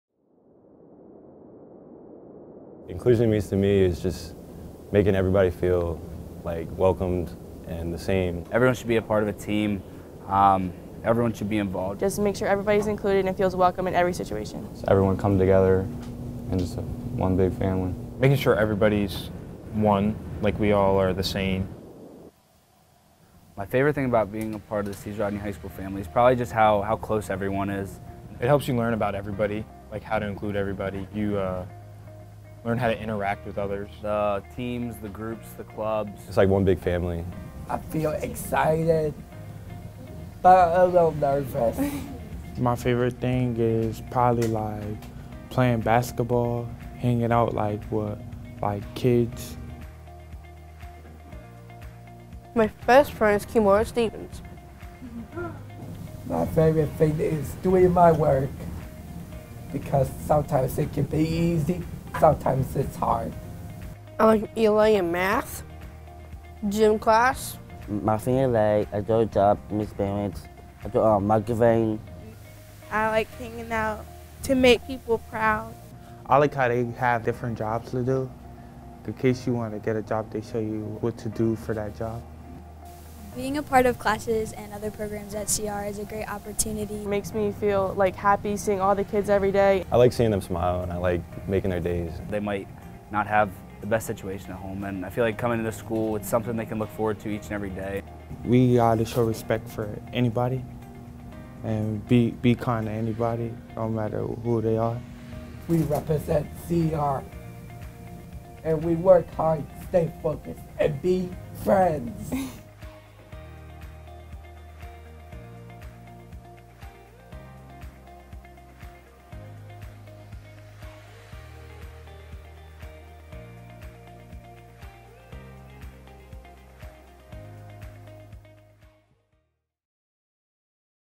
Spread the Word CRHS 2020
Join Caesar Rodney High School in spreading the word to end the word throughout the community!

Edited by ARVED ECE Senior: Isabella Andaya